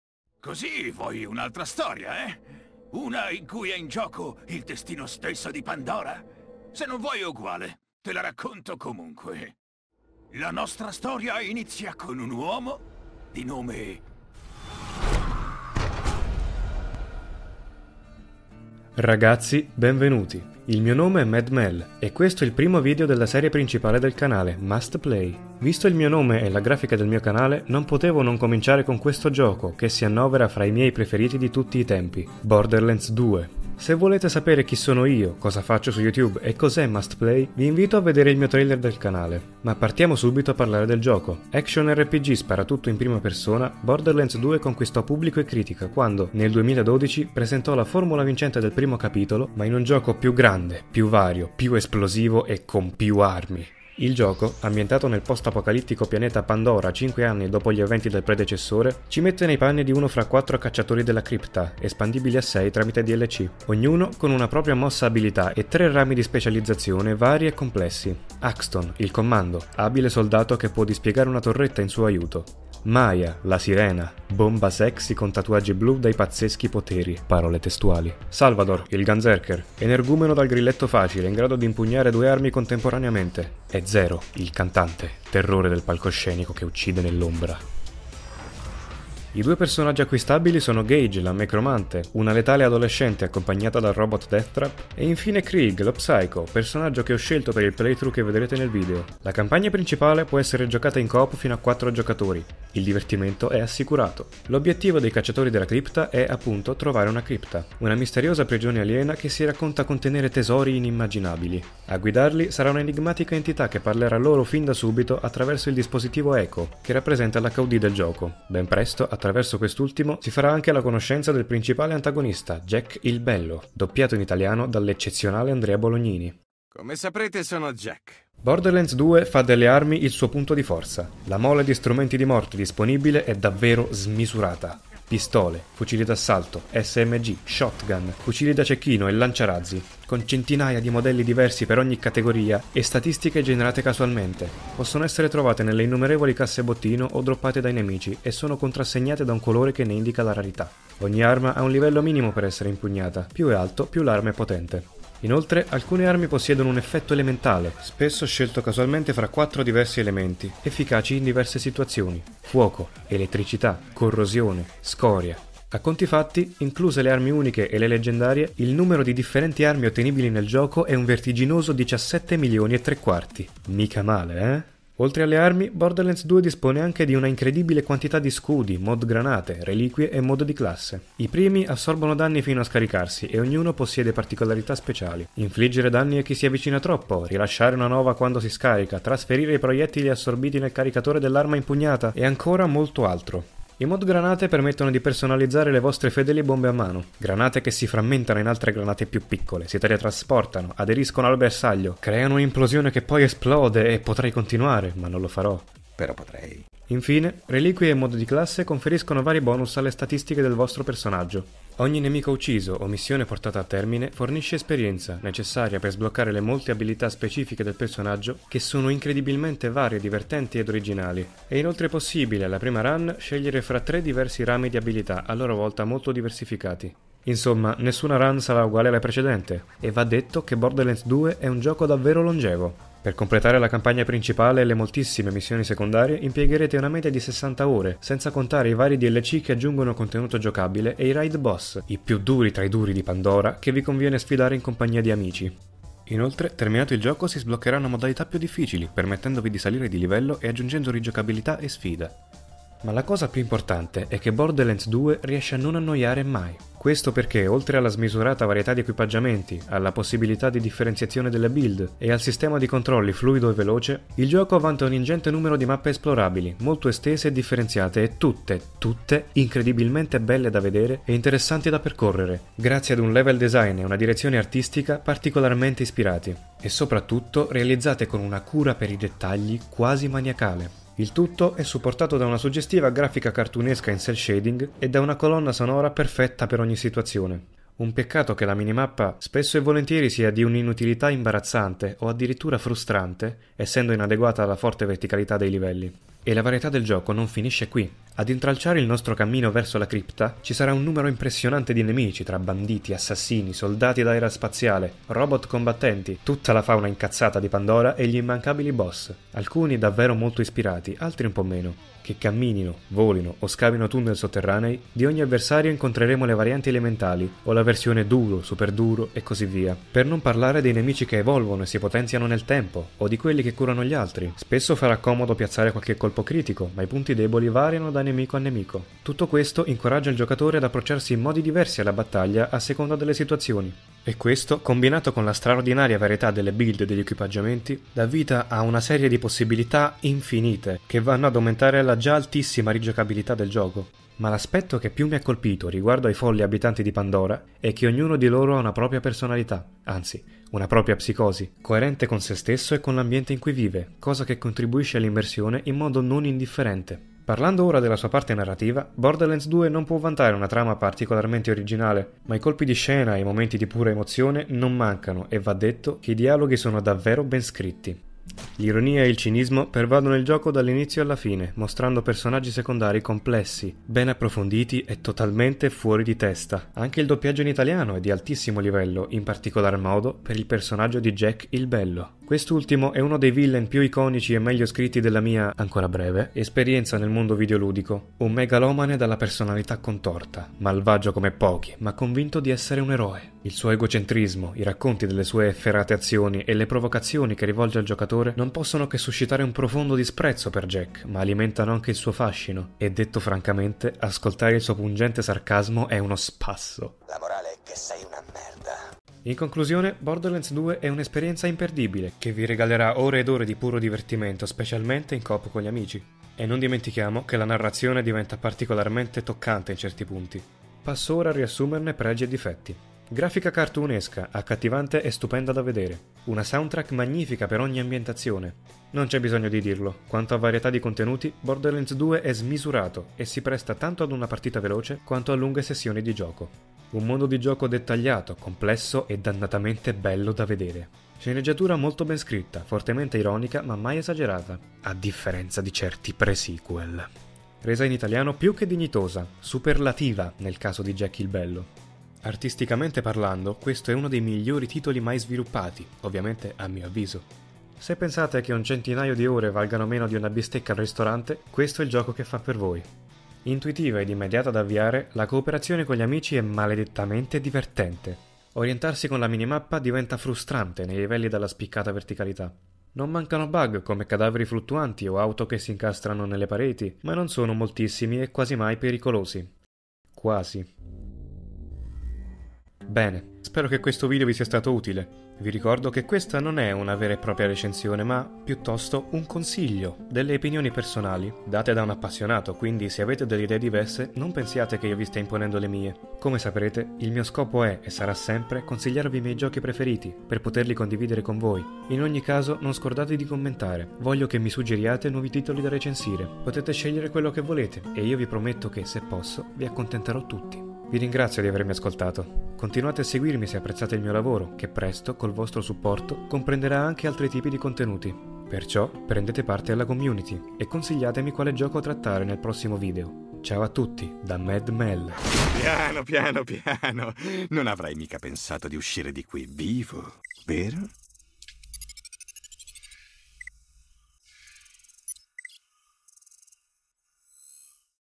MustPl4y: Borderlands 2 - Videorecensione [ITA - SUB ENG]
Primo video della serie principale, MustPl4y.
Borderlands 2 ha rappresentato un punto di svolta nella mia vita da gamer, e spero con questo video di aver invogliato quelli di voi che non lo conoscevano a dargli una possibilità.

Musica offerta da - Free Production Music